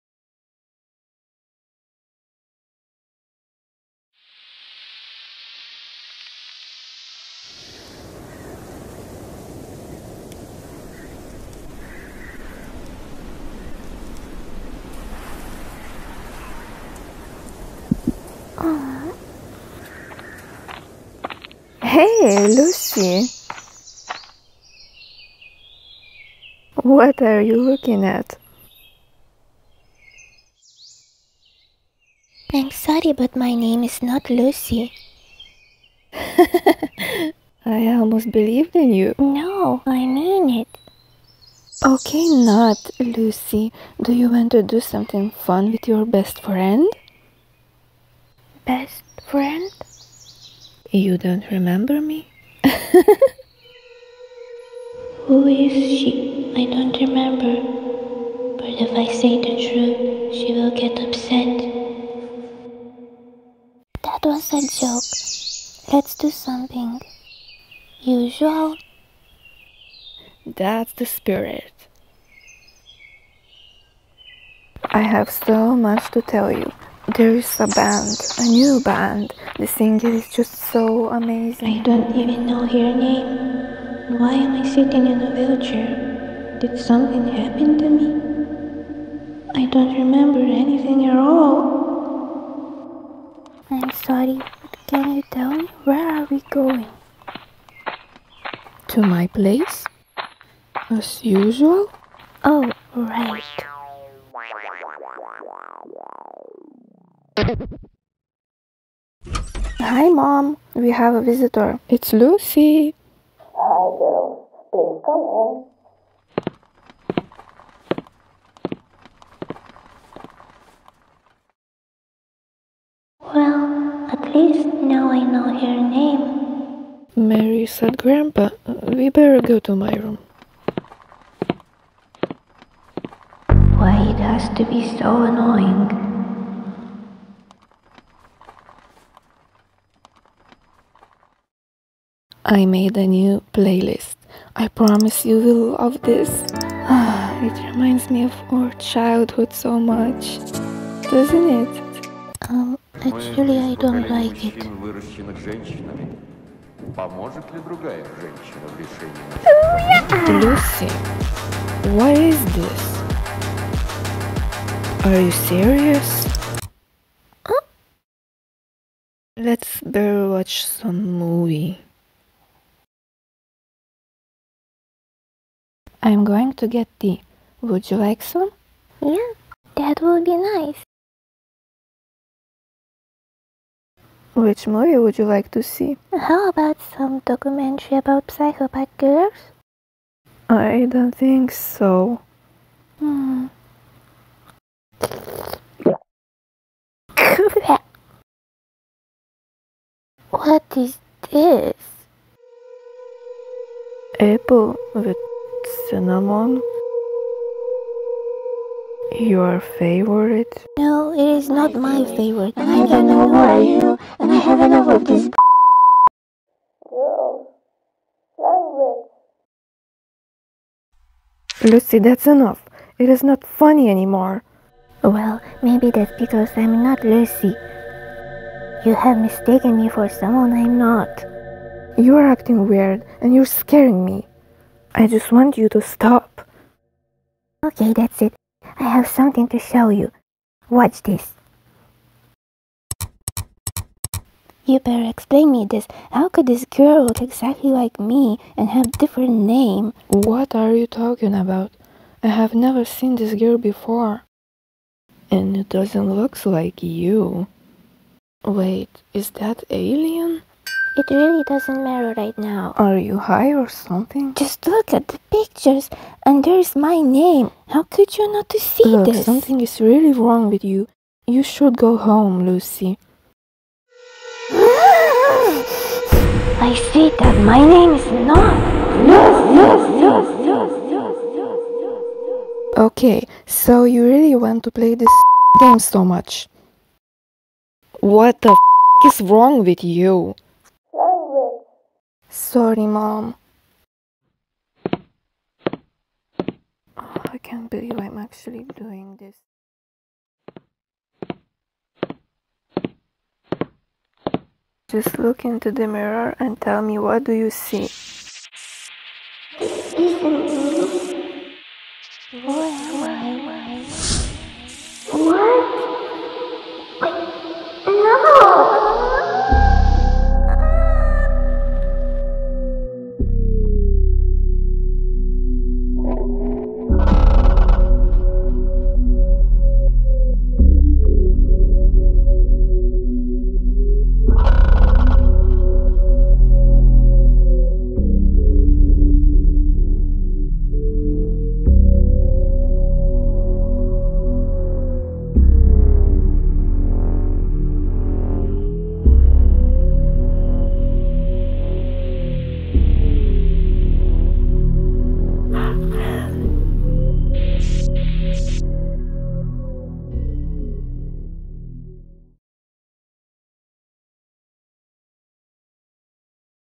Something's wrong with Lucy | Short Mystery Animation
You can turn on subtitles by pressing on the "CC" button at right corner on the video player.

About animation:

This is a story made out of my nightmare I had few months ago.
I could say this is mystery psychological short animated film mixed with comedy.
I recreated everything as I remember, places, rooms and those awkwardly funny moments which I had in that weird dream.
I saw everything in Lucy's eyes, so I made more scenes from the other point of view expecting to make it more filmic.